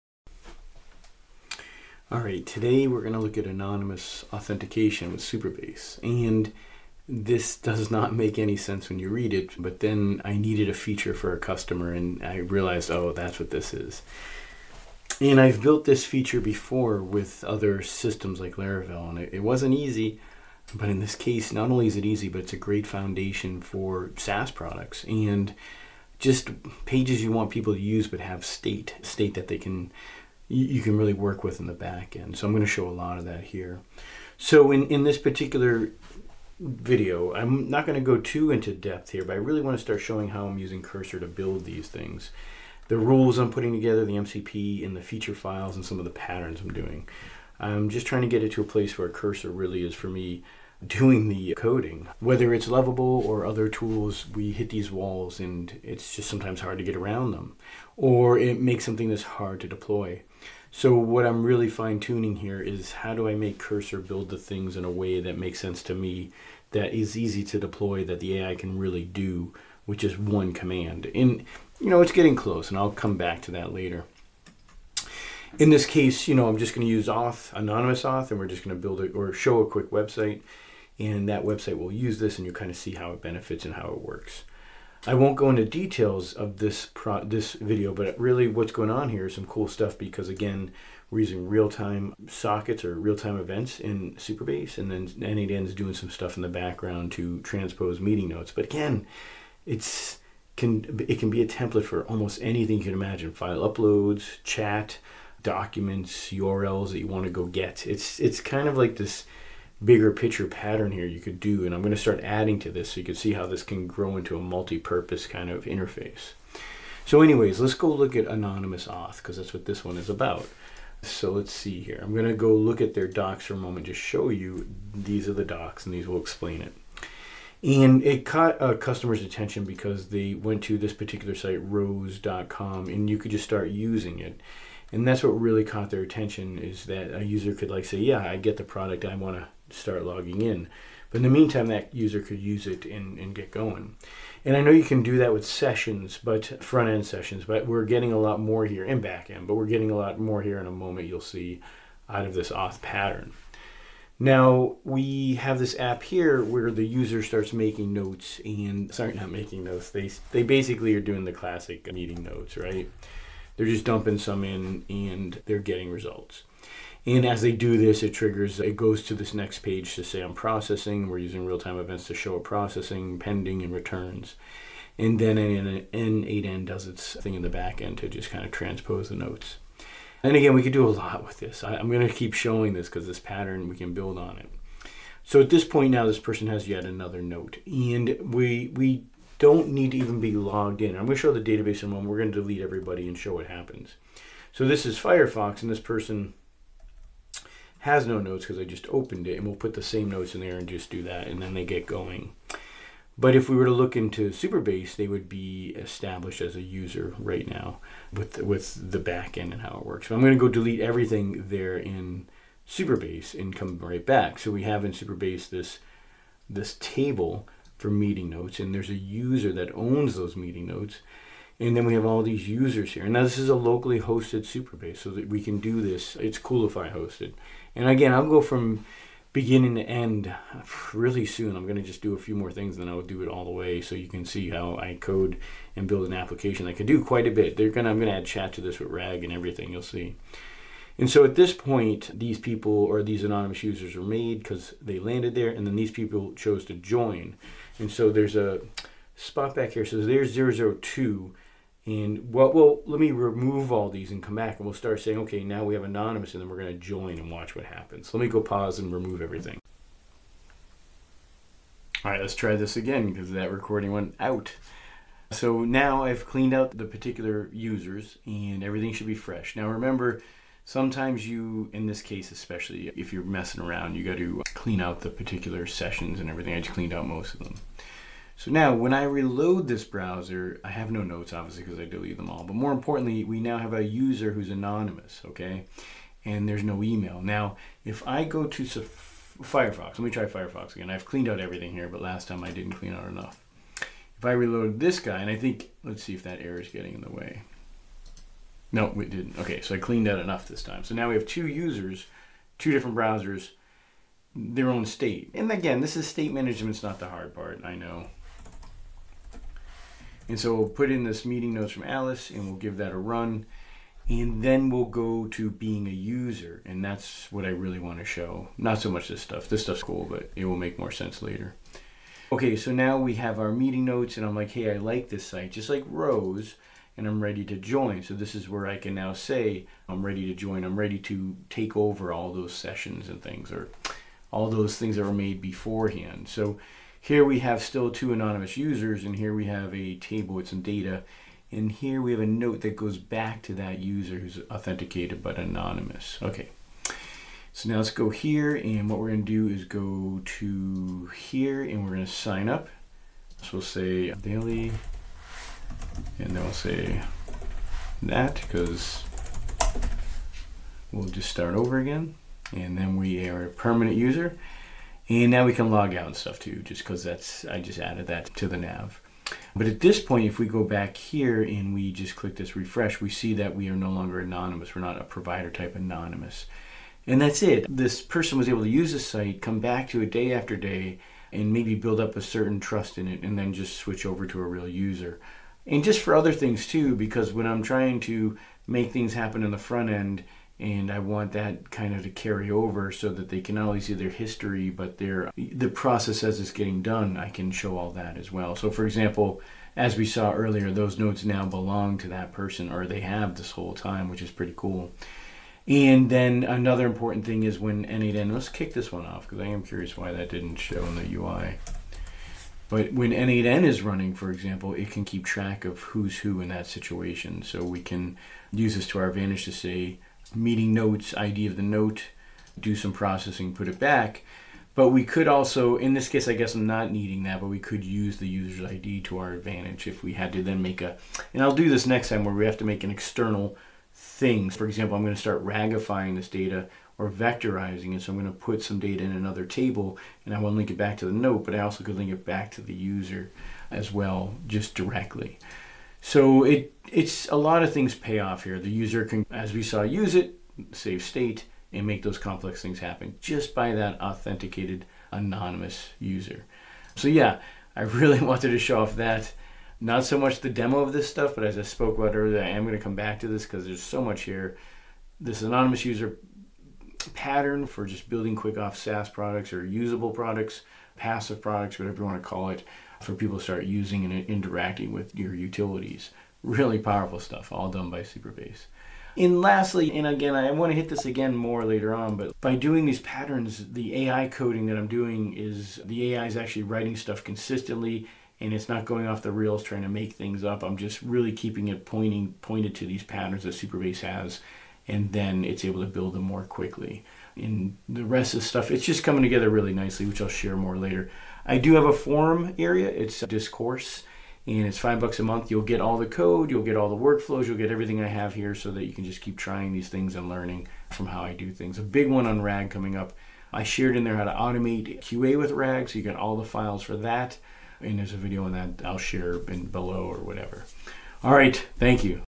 Supabase & Anonymous Auth
In this video, Alfred shows you how to do anonymous authentication with Supabase. A user can come to your app, explore it, and interact with it without an account. When they decide to register, they can do so without losing any of their previous interactions or data.

## Chapters
[00:00] Introduction to Anonymous Authentication
[01:00] Setting Up and Using Supabase
[02:00] Demonstrating User State Management
[03:00] Transitioning from Anonymous to Registered Users
[05:00] Real-Time Events and Backend Processing
[07:00] Practical Applications and Future Developments